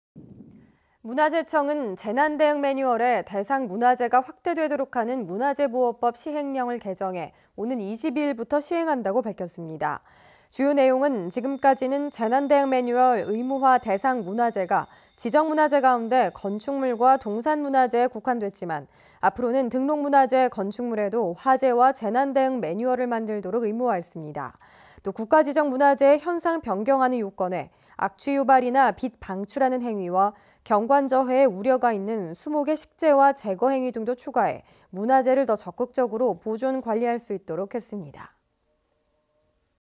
[포항MBC뉴스]문화재 보호법 개정
문화재청은 재난대응 매뉴얼의 대상 문화재가 확대되도록 하는 '문화재보호법'시행령을
개정해 오는 22일부터 시행한다고 밝혔습니다
           ◀ＥＮＤ▶

주요 내용은 지금까지는
재난대응매뉴얼 의무화 대상 문화재가
지정문화재 가운데 건축물과 동산문화재에
국한됐지만,앞으로는 등록문화재 건축물에도
화재와 재난대응 매뉴얼을 만들도록
의무화 했습니다

또 국가지정문화재의 현상변경하는 요건에
'악취 유발이나 빛 방출하는 행위’와
‘경관 저해의 우려가 있는 수목의 식재와 제거 행위’ 등도 추가해 문화재를 더 적극적으로
보존 관리할 수 있도록 했습니다.